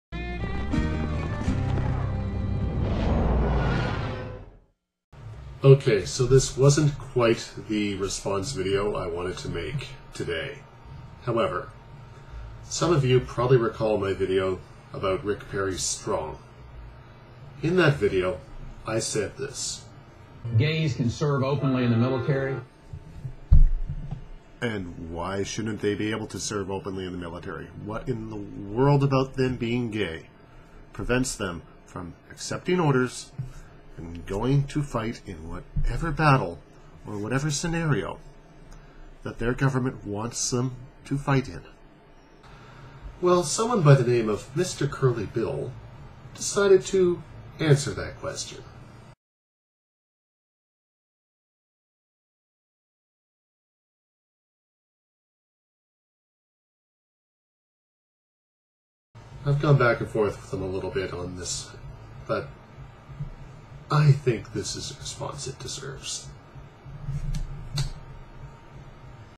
An Appropriate Response to MrCurlyBill
I may have been feeding a troll, but damn, if that's your best argument, give it up dude.

Your arguments are the worst kind of nonsense victim blaming and do not apply in ANY other scenario, and therefore they do not apply here.